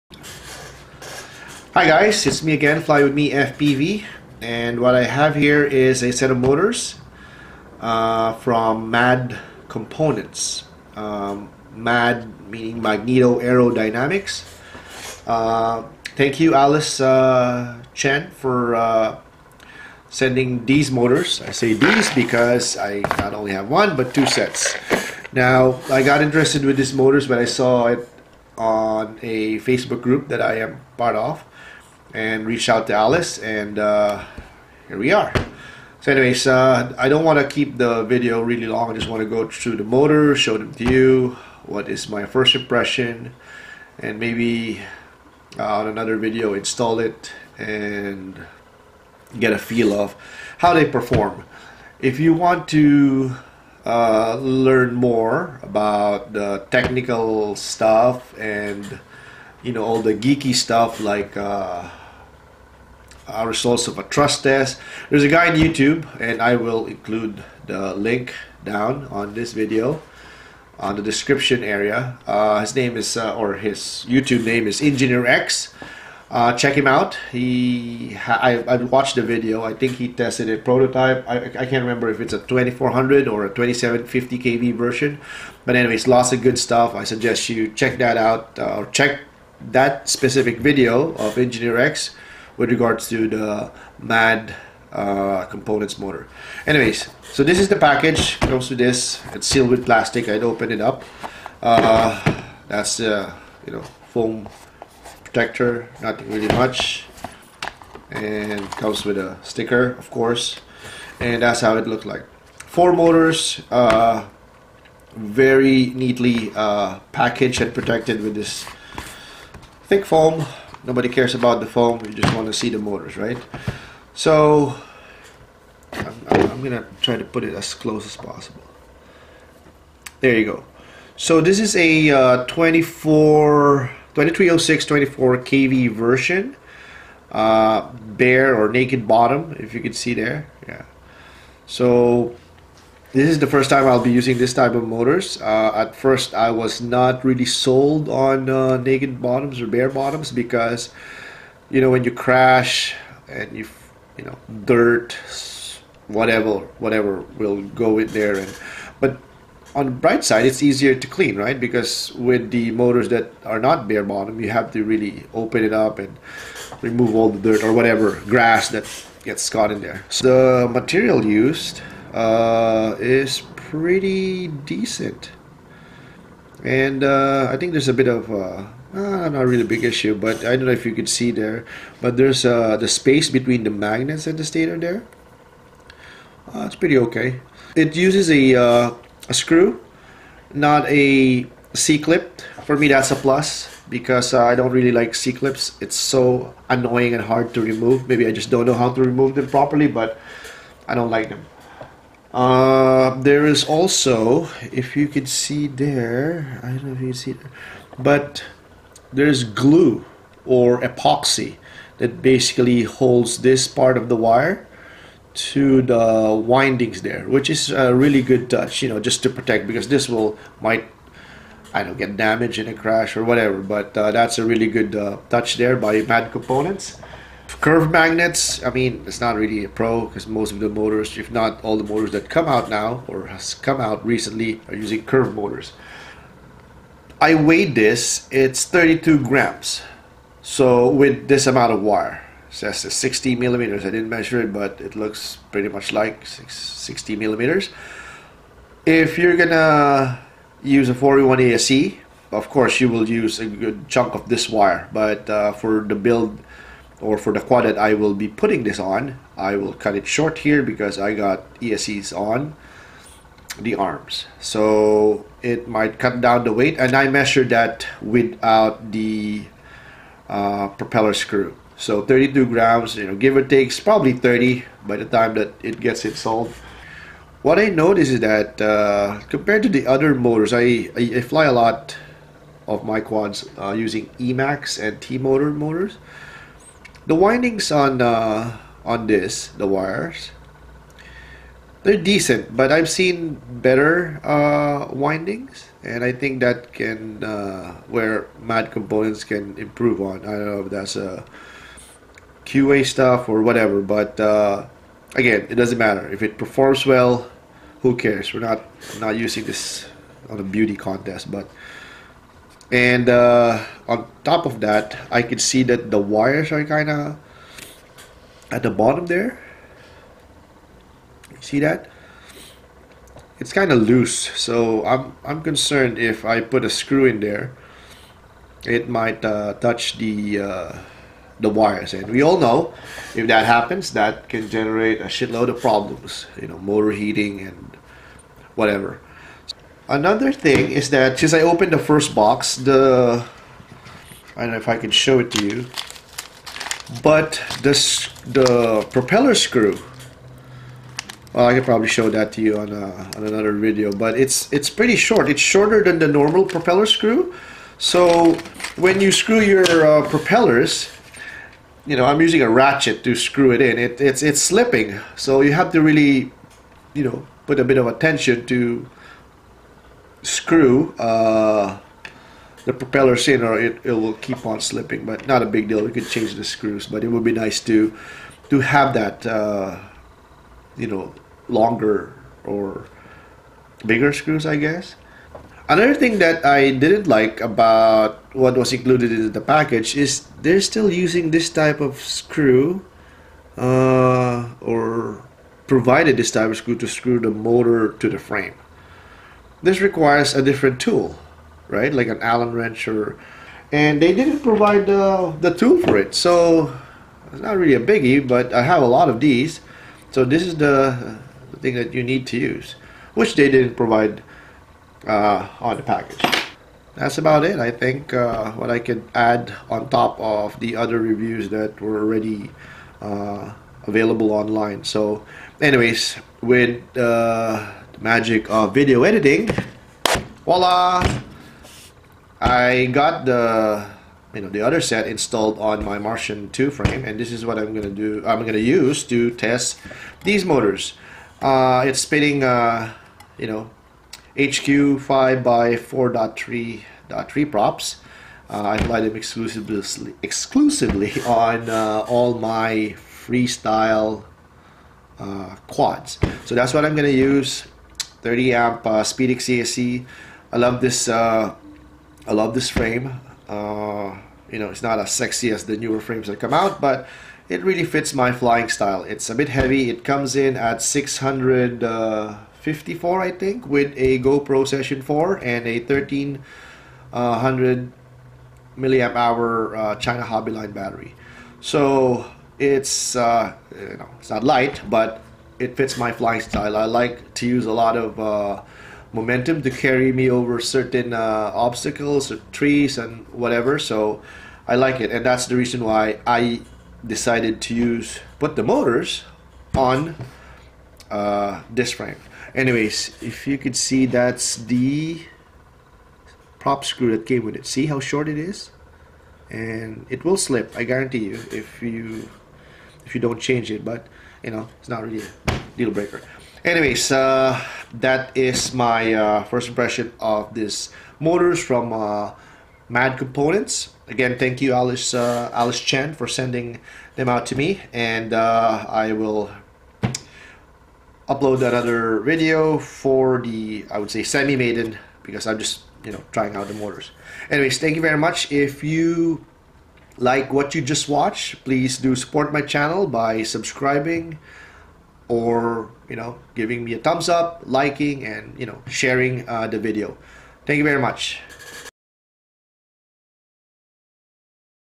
First impression - MAD2306 2400kv motors from MAD Components
Got this MAD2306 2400kv motors from MAD Components. This video is about what I like, what I don't like and what I think MAD Components can improve on with regards to the design and build quality. For all the technical and geeky stuff check out this Youtube video by EngineerX